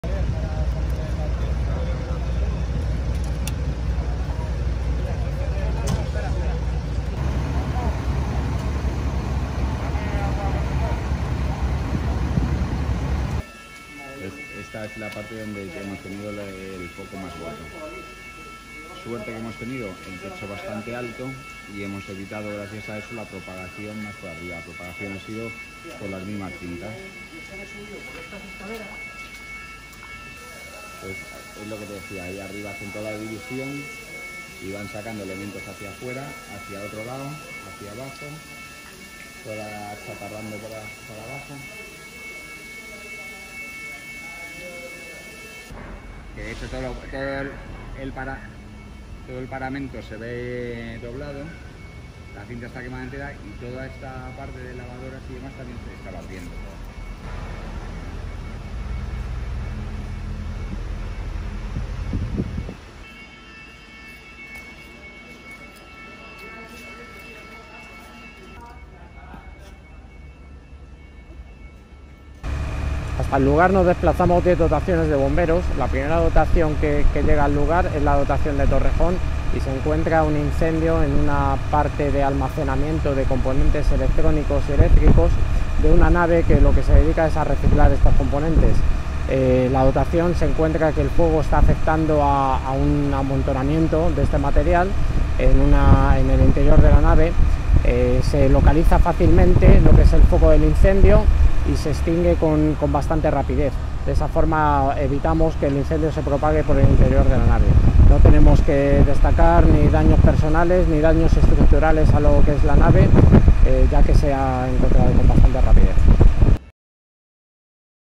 14.7,20 incendio industrial Torrejón de Ardoz
10 dotaciones BomberosCM trabajan en la extinción